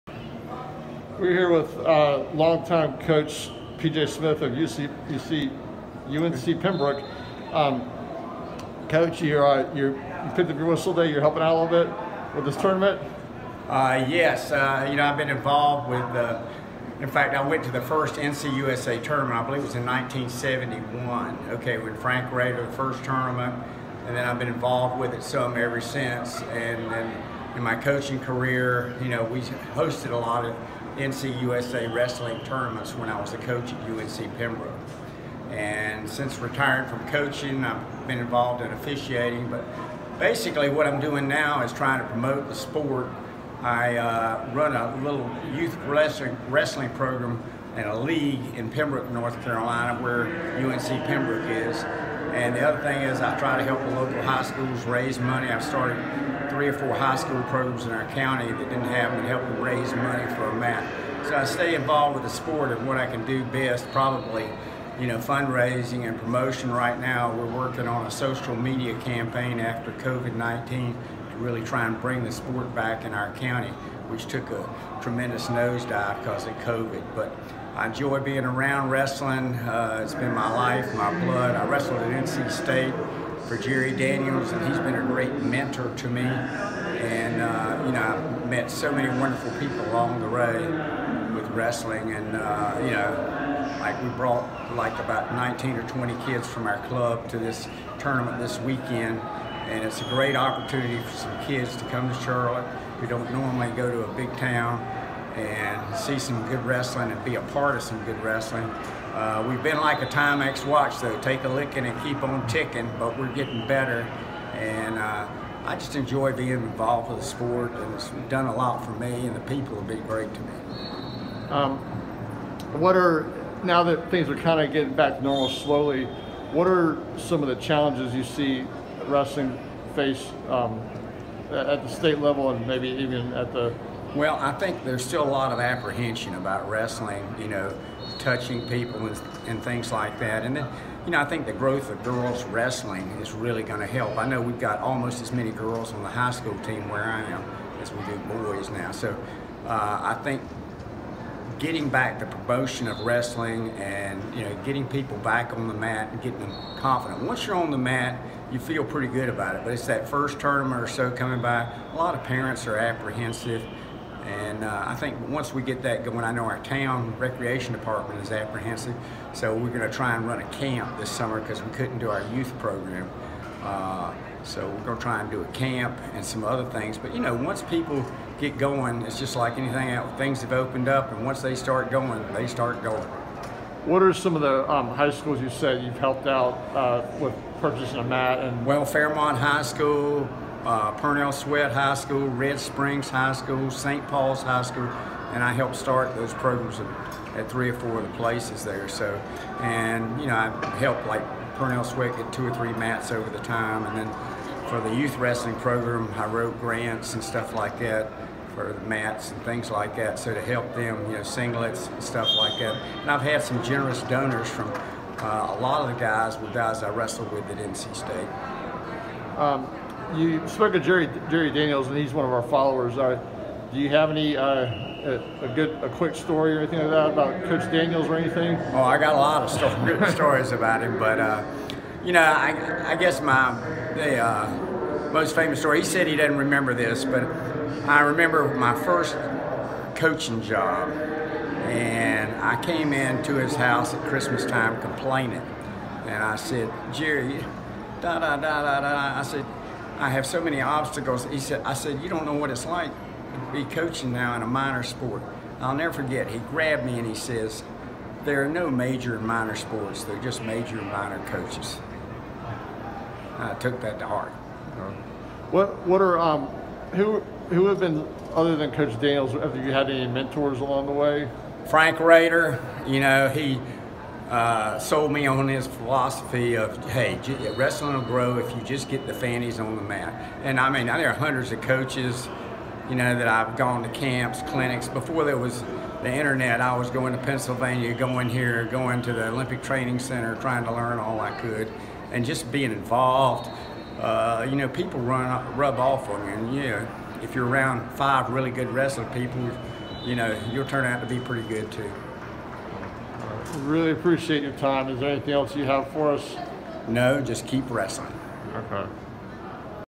PJ Smith interview
Spoke with former UNCP coach PJ Smith. He talks about his love of the sport and his passion to promote, grow the sport in his community(Robeson Co) and across North Carolina. We are truly blessed to have such  a great ambassador for wrestling like PJ in our state.